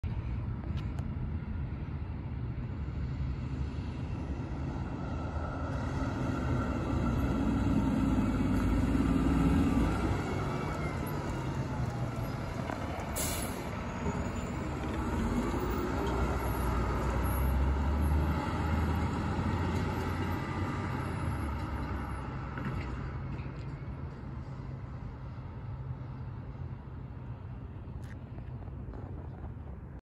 OCFA Engine 22 returning
Here you see Engine 22 returning to station after going to a medical aid